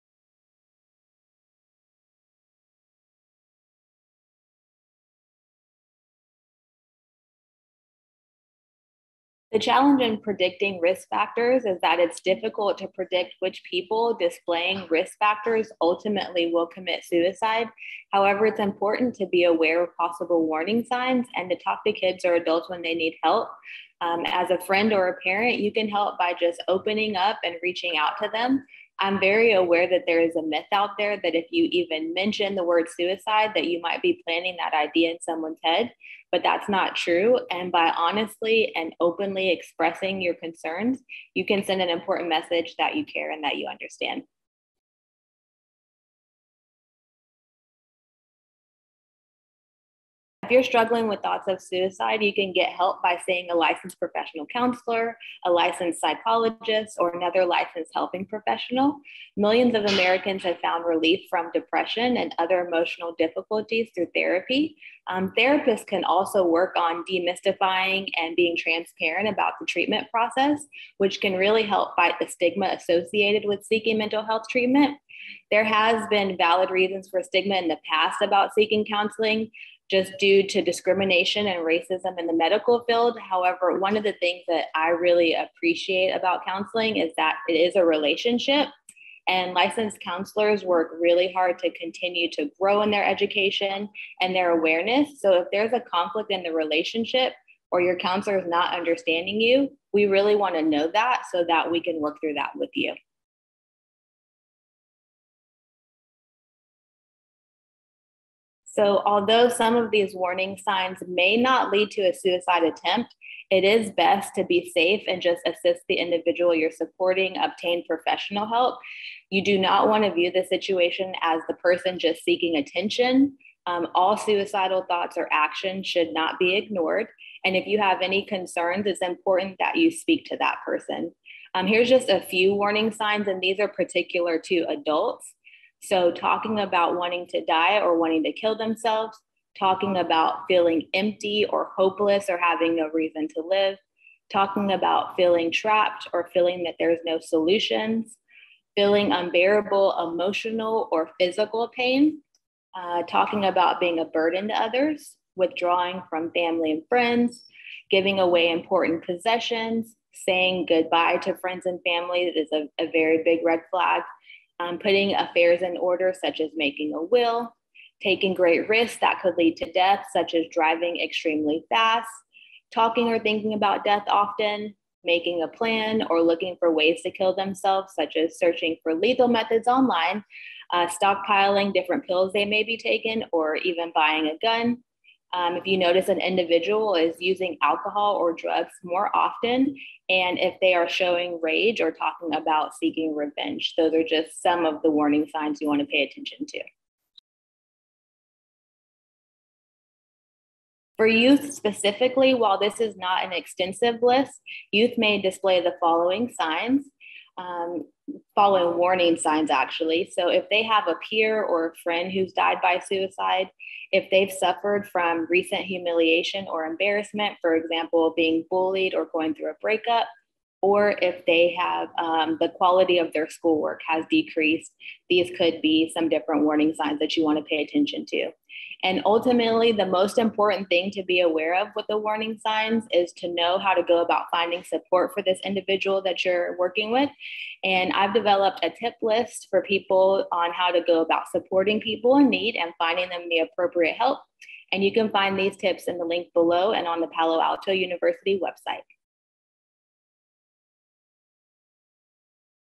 Preventing Suicide: The Warning Signs
Professor LaToya Smith answers four questions about detecting the warning signs of suicide.

What are the challenges in predicting risk factors an individual at risk might display?

How can mental health professionals help people who are struggling with thoughts of suicide?

What are some of the warning signs of people who are at risk of committing suicide?

Are there specific warning signs that pertain to youth?